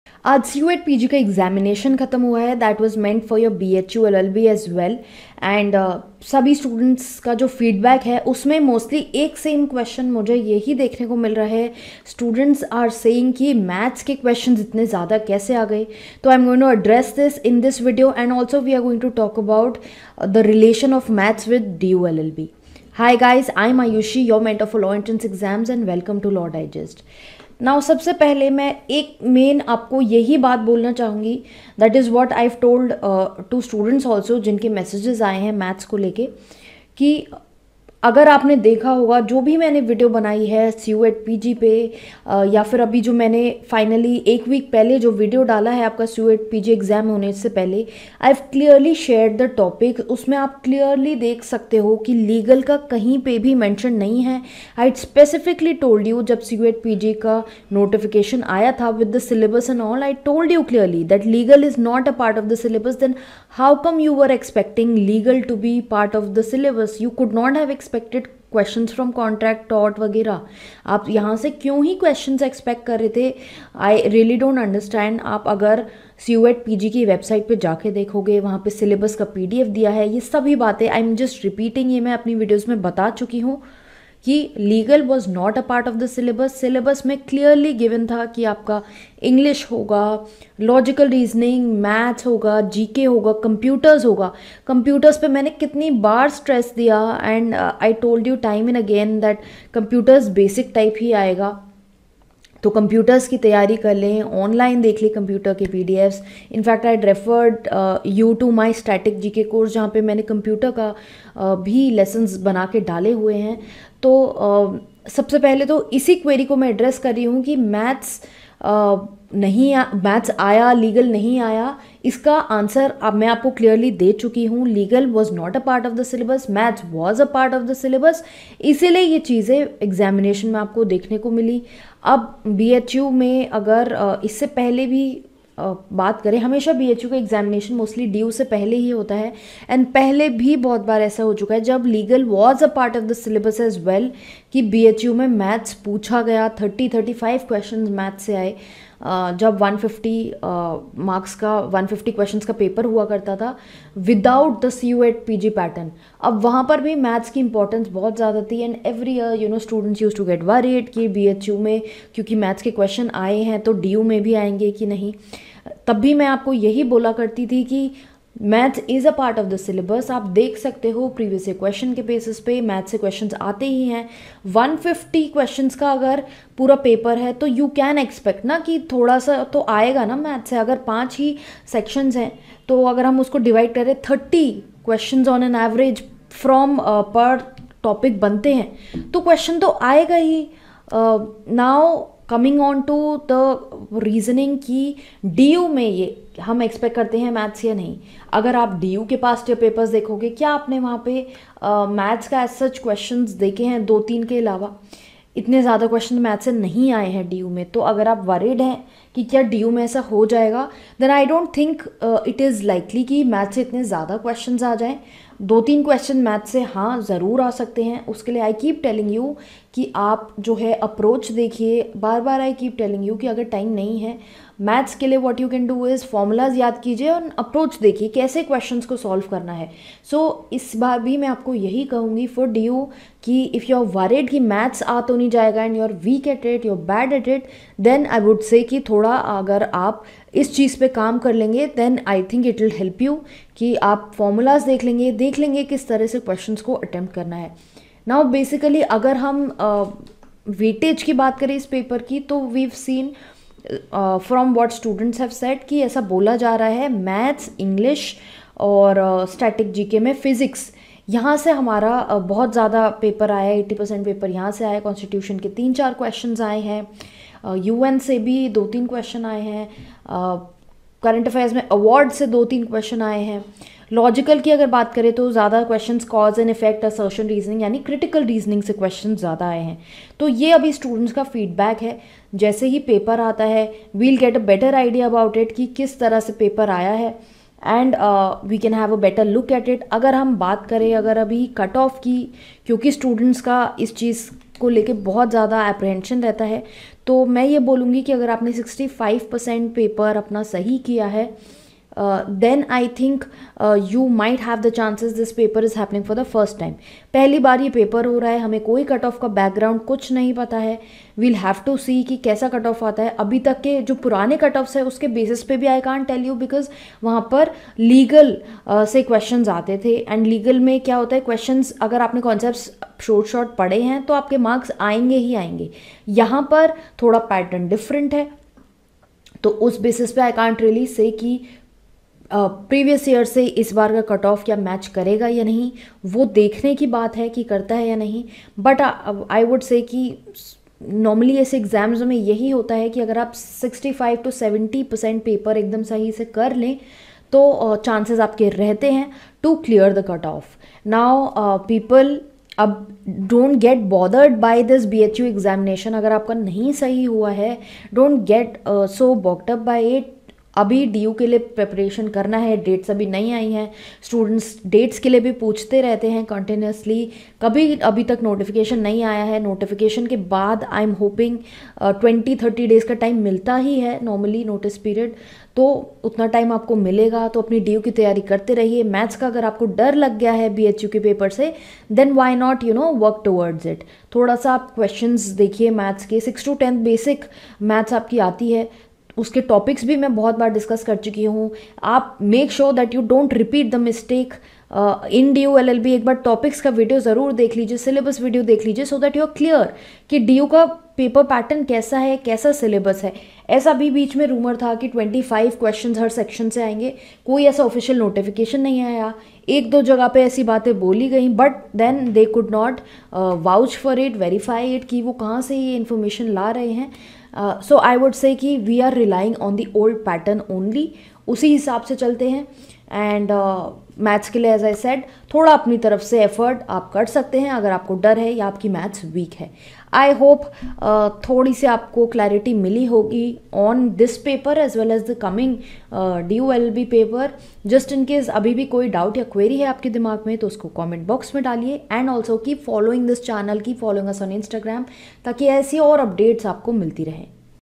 BHU LLB CUET PG 2022 Exam Analysis | Expected Cut Off
You can also get in touch with me on the following social platforms: